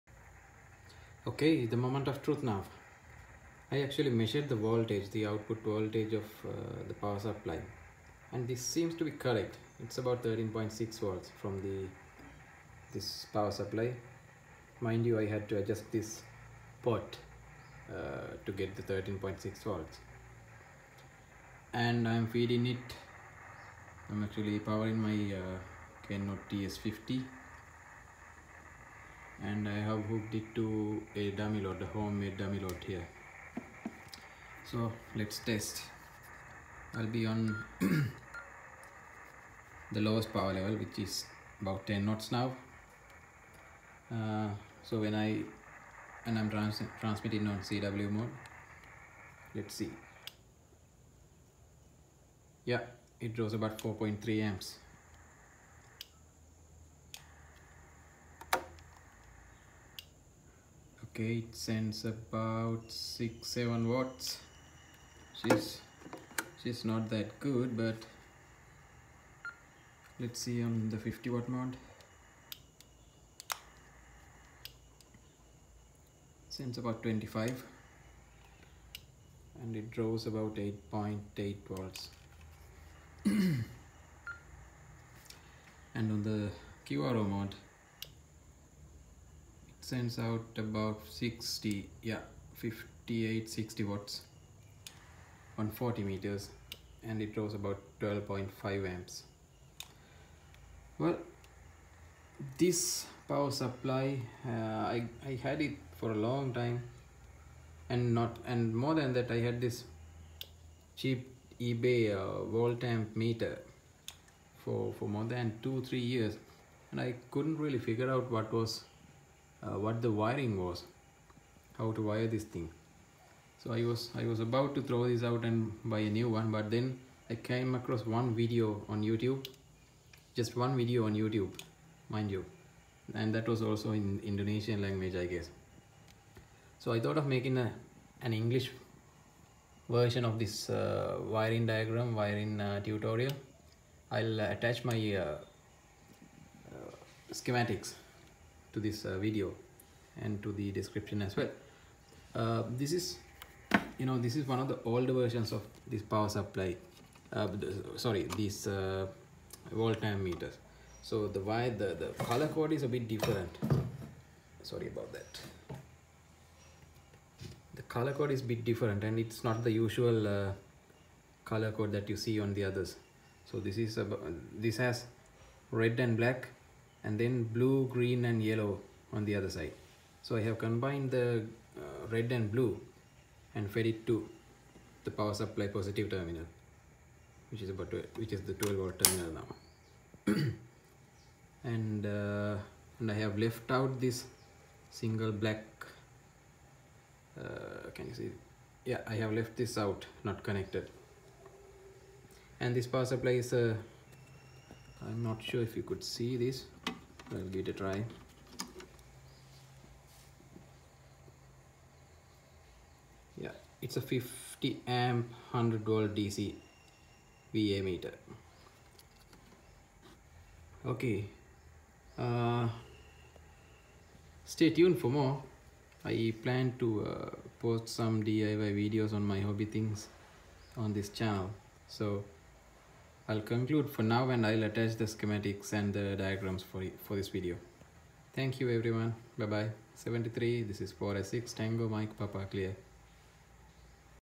DC 100V 50A ebay Digital Volt Amp meter (old version circa 2017) Wiring
I was tempted to make a video on how to wire this older version of the DC 100V 50A Digital Volt-Amp meters as there was no clear tutorial I could find on the web. Of course, there are tonnes of videos on the newer version.

Link to the wiring diagram attached to the bottom of te description.

The wire colour code is different for this one( RED-BLACK, YELLOW, GREEN, BLUE). This version does not come with an inbuilt current shunt. Thus I am using a 50A, 75mV shunt here.

I will not bore you with simple steps like how I soldered and the wires etc. In fact, this was done temporarily. I will make a proper enclosure and complete the project later.

I am not even sure if this version of the meters is still in production.